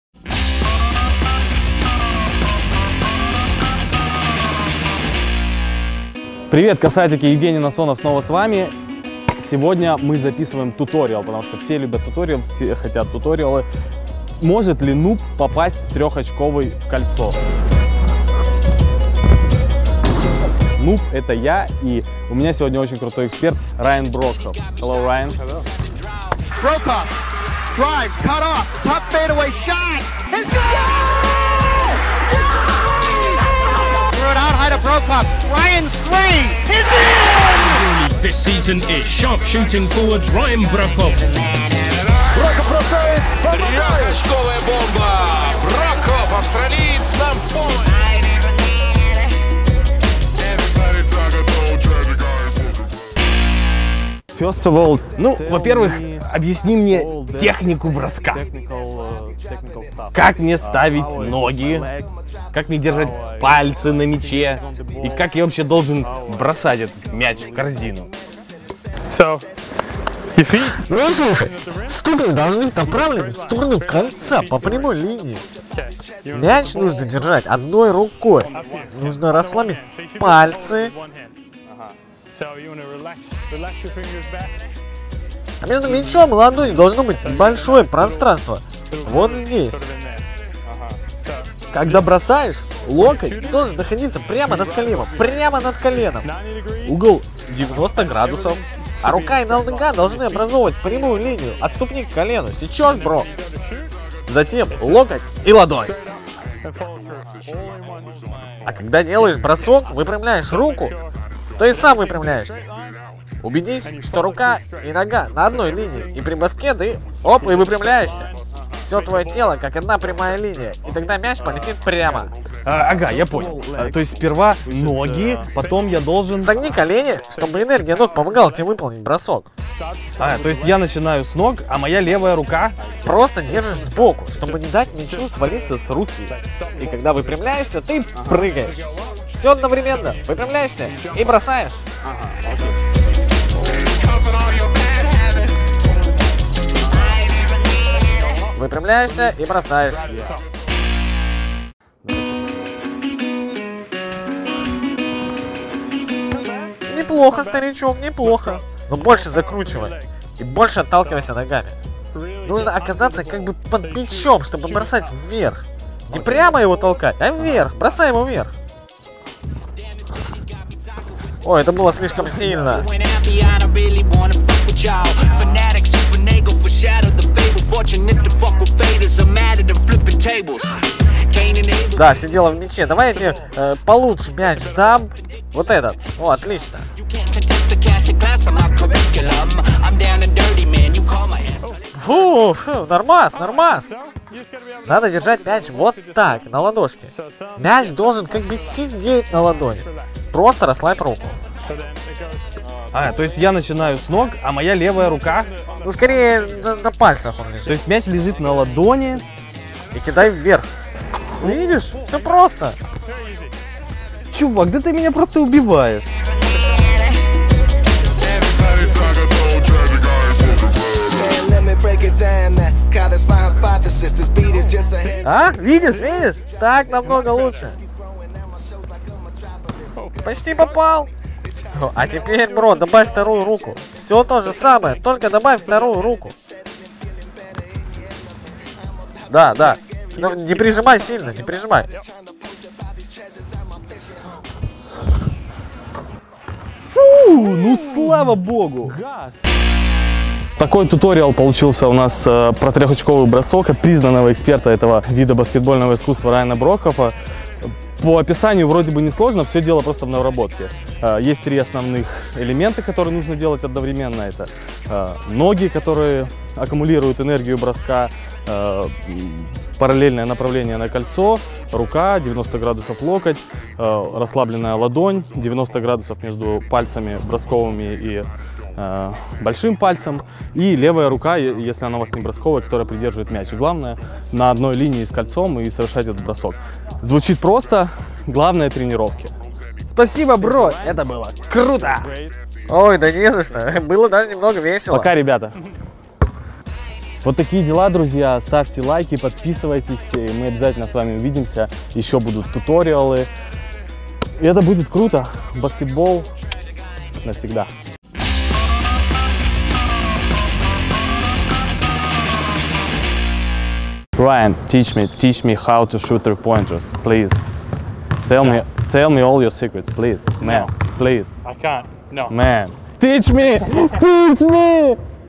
МОЖЕТ ЛИ НУБ ЗАБИТЬ ТРЕХОЧКОВЫЙ? | УРОК ОТ БАСКЕТБОЛЬНОГО СНАЙПЕРА
Что если нуб, не умеющий играть, захочет попасть в корзину броском из-за дуги? Это вообще возможно? Да, если учить будет Райан Брокхофф — один из лучших снайперов Европы.